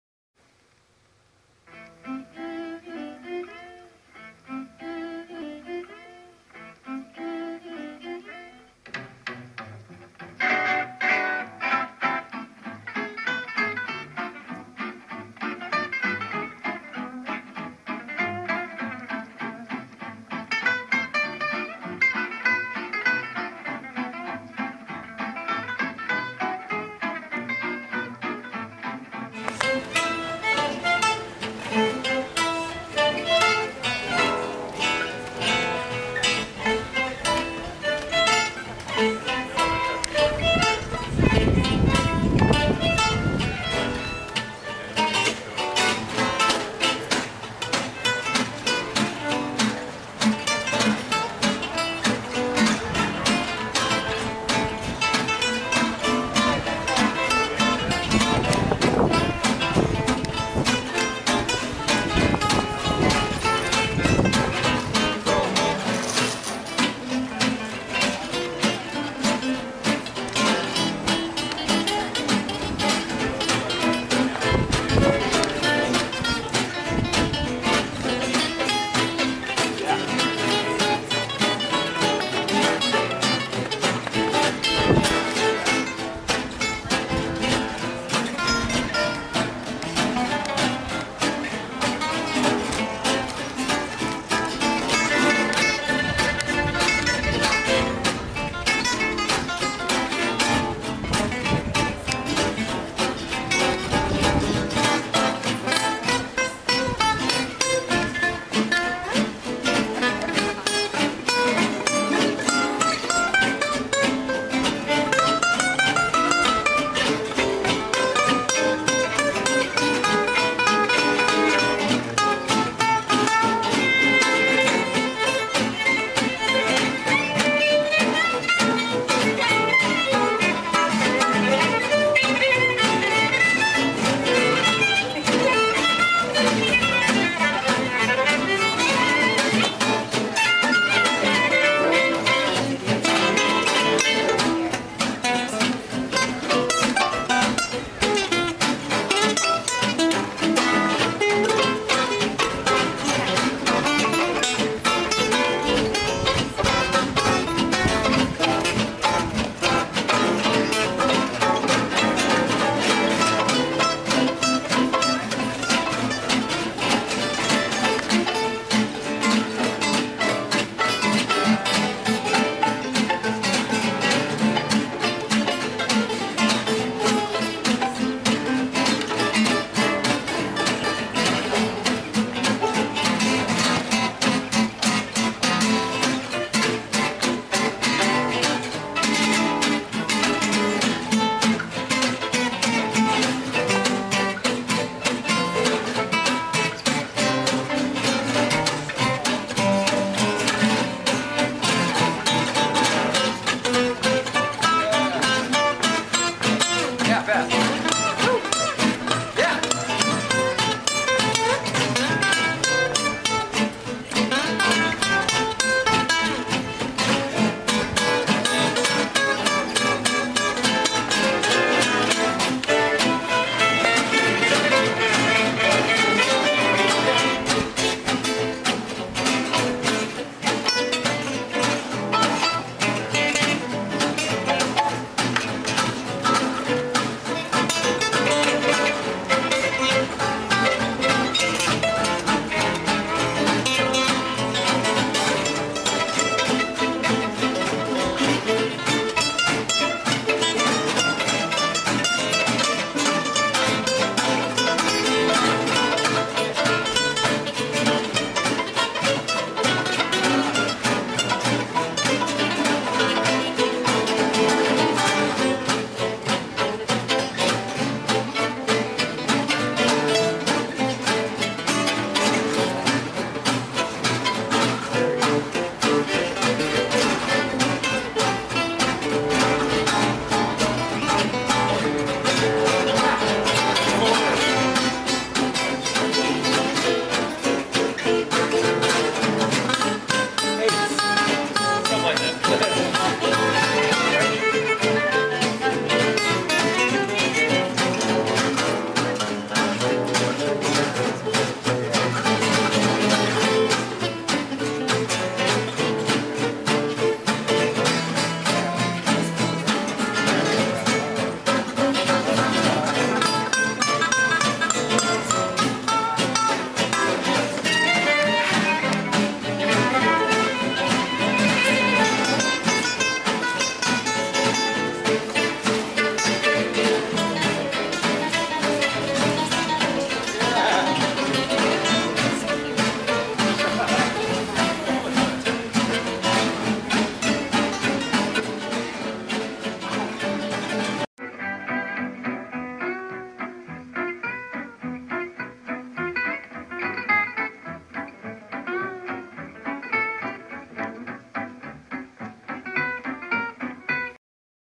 DjangoFest Northwest 2010 #5
Music inspired by Django Reinhardt. played in Langley, WA (Whidbey Island)